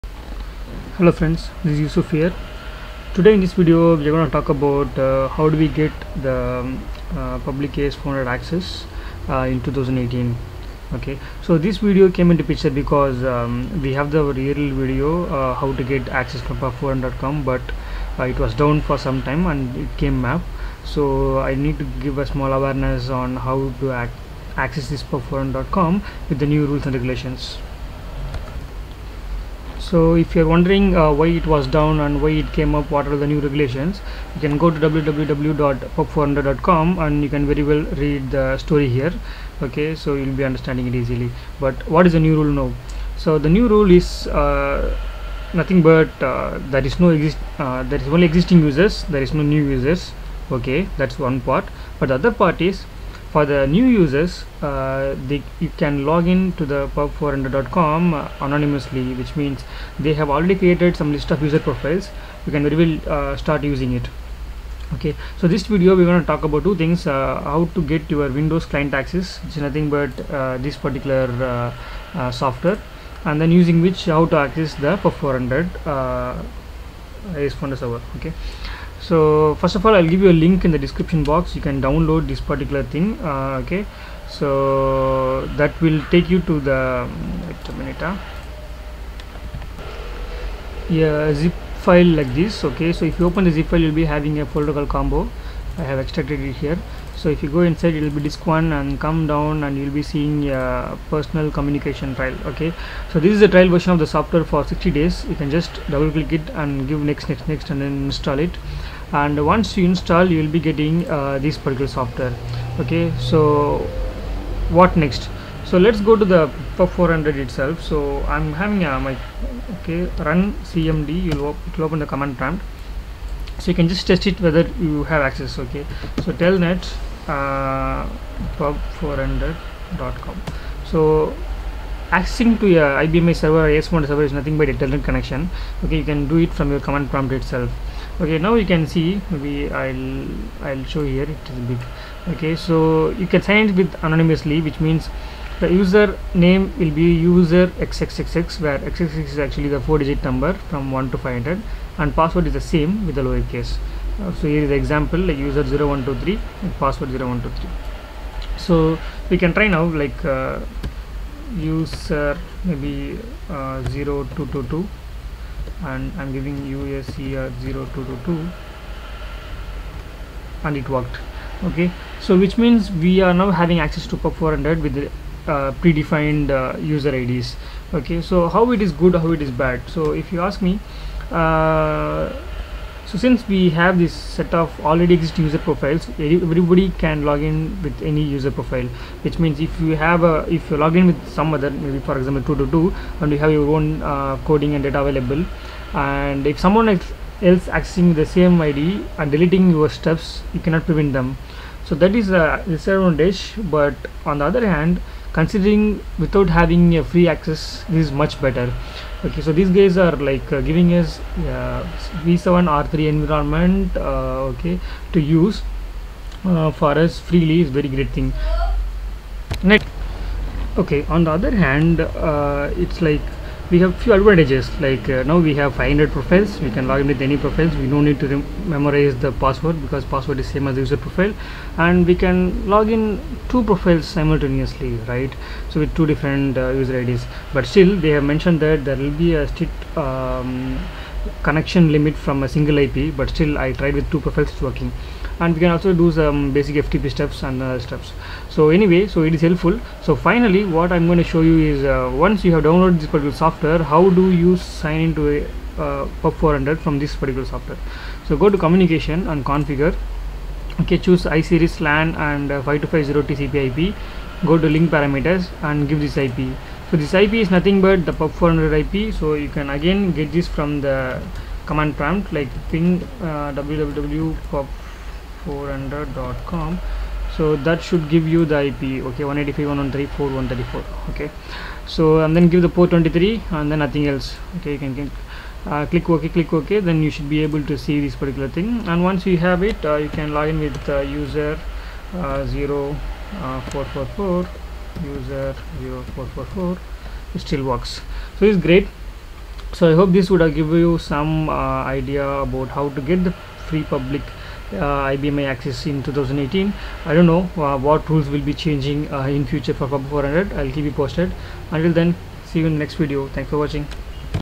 Free IBM i (AS400) login using PUB400 in 2018 | yusy4code
This video explains how to login into pub400 using free user profile.

Link to download IBM client access for Windows